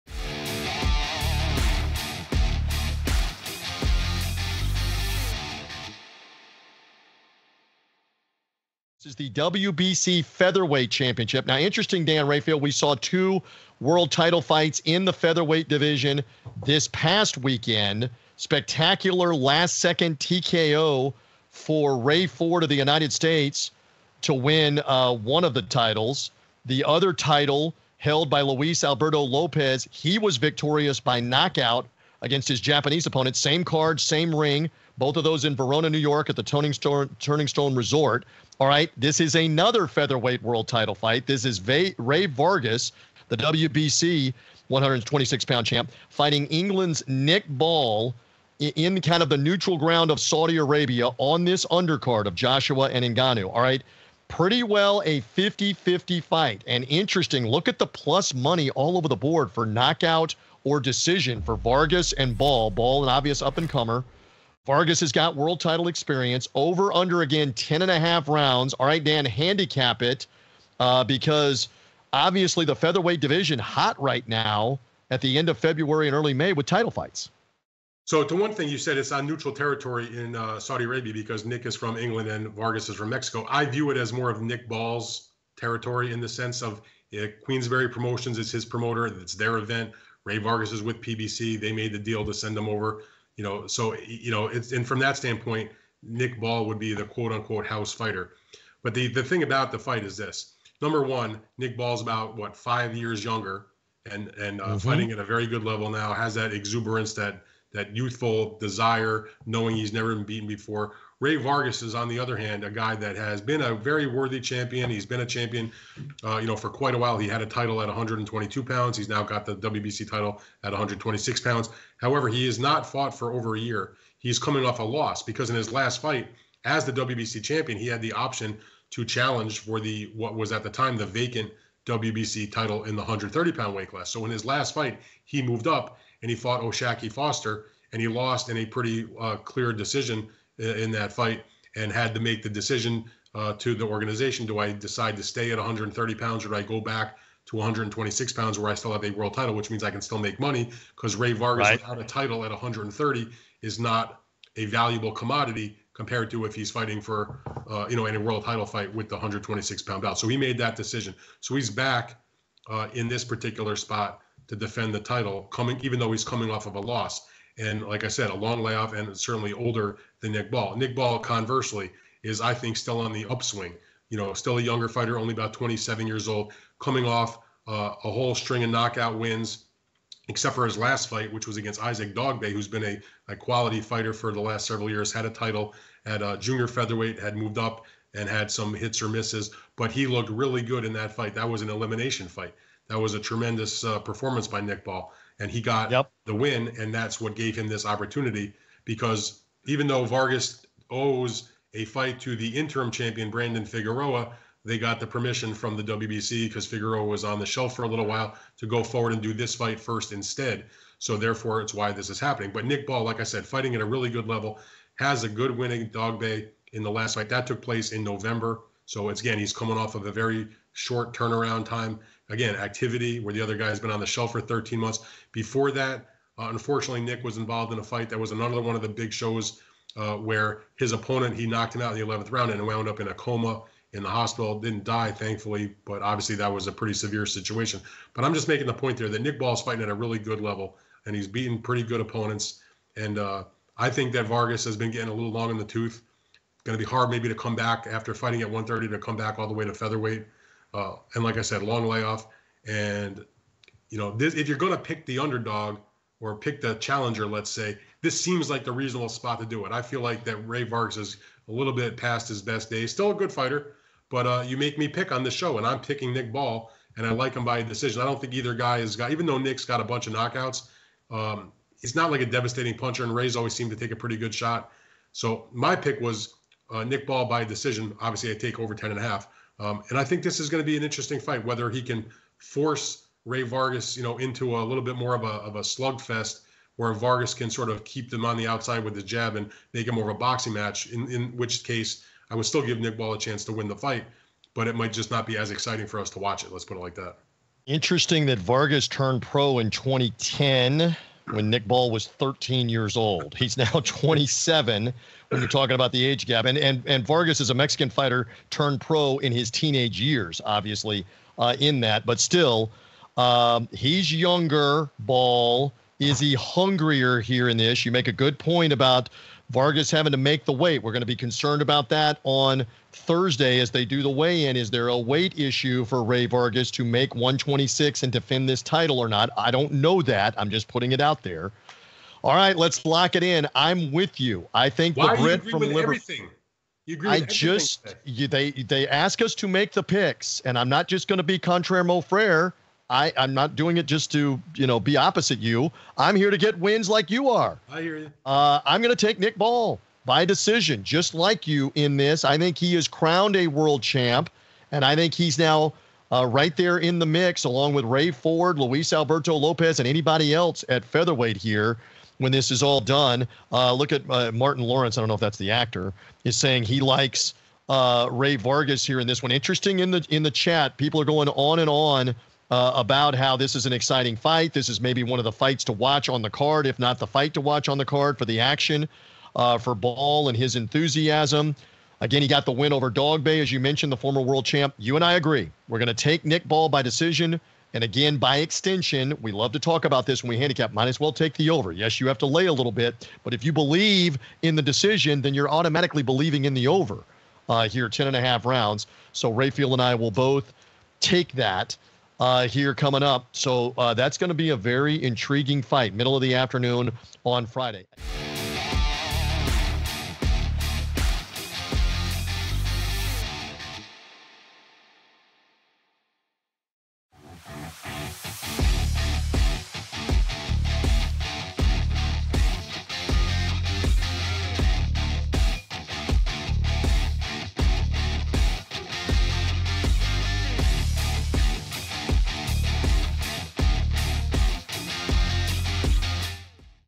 Rey Vargas vs Nick Ball | Boxing Expert Predictions, Boxing Picks & Best Bets
Rey Vargas vs Nick Ball boxing fight predictions for March 8th, 2024

This fight presents a classic stylistic matchup of the boxer versus the pressure fighter. Vargas's technical skills and ability to control the range make him the favorite on paper. However, Ball's aggression and pressure could pose significant challenges, especially if he can successfully close the distance and force Vargas into a more uncomfortable, close-range fight.

TJ Rives and Dan Rafael preview the fight between Rey Vargas vs Nick Ball. They'll also go over the latest boxing betting trends, best value boxing odds available, and share their boxing expert predictions and boxing picks.

Connect with our betting experts on X:
Dan Rafael: @DanRafael1